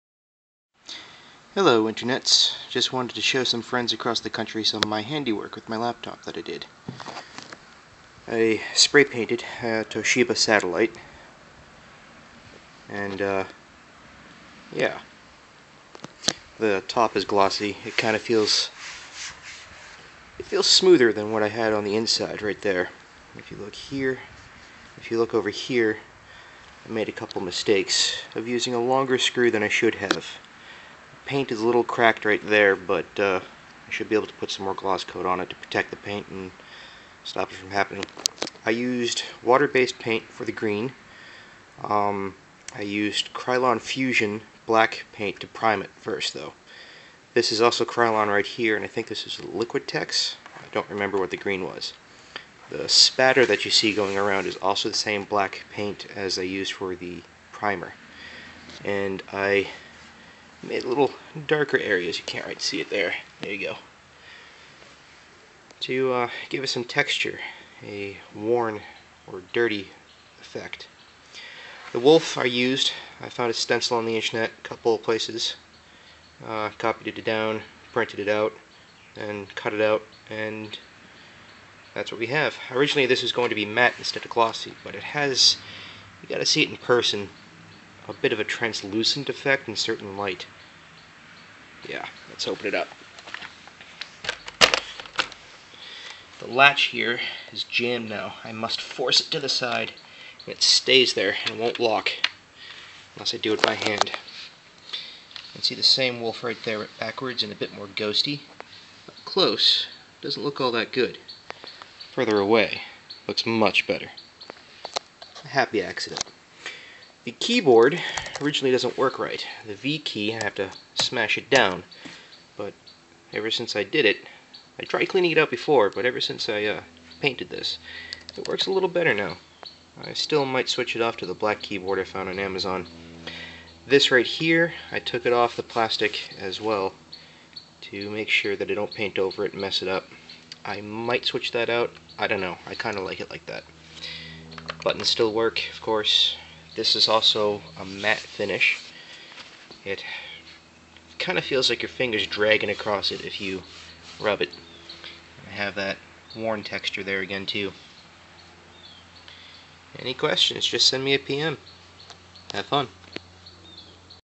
Painted Toshiba Laptop (named it "Sah")
So I painted my laptop and this is the result. EXTREMELY happy with how it turned out.

I wanted a laptop that was MINE, so many of the things about this laptop are reflections of me. The green is my favorite color. The red stripes are representing my birthday, Dec 24th (Green and Red). The wolf is twofold: Companionship and astronomy.

Companionship is because it is my laptop "companion". Astronomy is because of  research I did. I thought Canis Majoris (the one with the brightest start in the sky, Sirius, aka, The Dog Star) was called Sah by the Egyptians. Instead, Sah is what we call Orion today, which is my favorite constellation anyway.

One flaw I forgot to mention: The status lights on the front no longer shine through the paint. Don't care too much about this, as I know then it's on or off and I know when it's accessing the hard drive from the slight noise and vibrations it makes anyway. The only worry is over charging the battery, but a little attention paid to it will fix that.